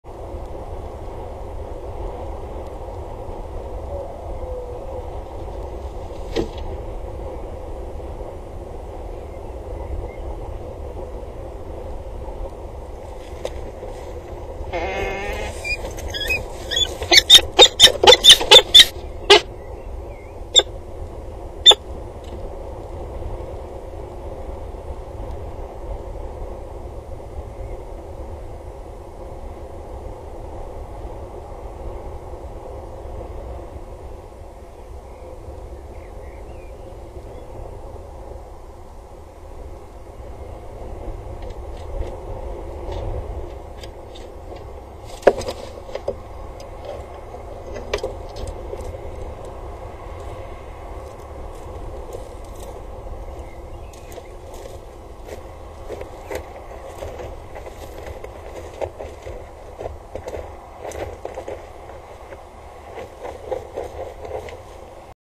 WatertorenAxelBinnen 2016 04 27 211403 SV wissel Alexia komt voor de nacht 3 eitjes
21.14 uur. Alex hoort Alexia en gaat van het nest. Alexia neemt over voor de nacht. Duidelijk nog 3 eitjes.